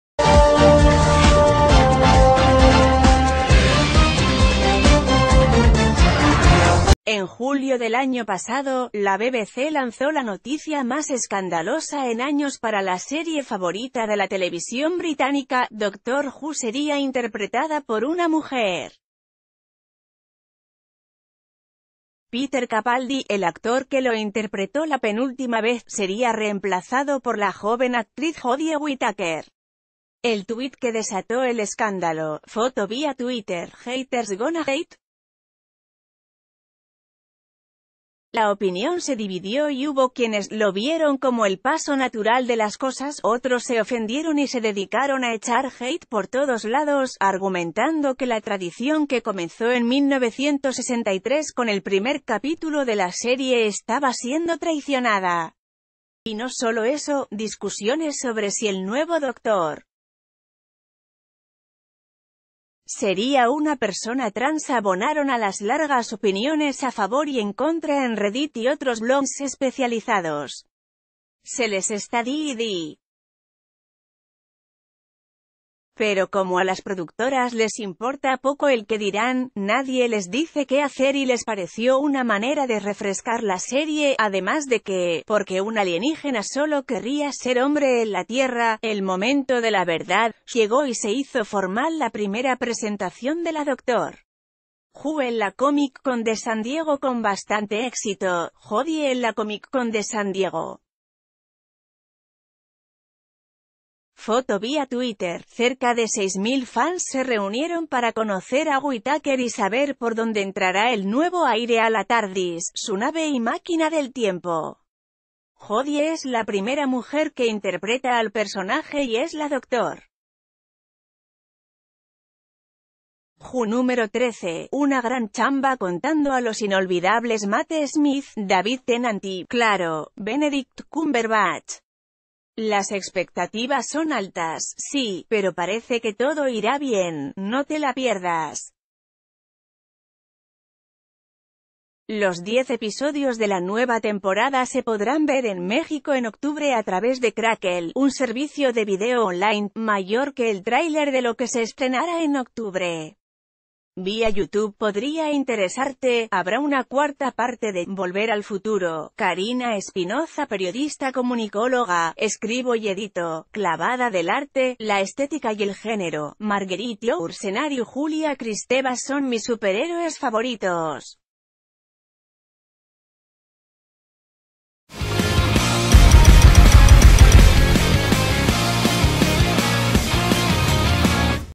Inevitable: la nueva Dr. Who sí gusta
En julio del año pasado, la BBC lanzó la noticia más escandalosa en años para la serie favorita de la televisión británica: Dr. Who sería interpretada por una mujer. Peter Capaldi, el actor que lo interpretó la penúltima vez, sería reemplazado por la joven actriz Jodie Whittaker. El tuit que desató ...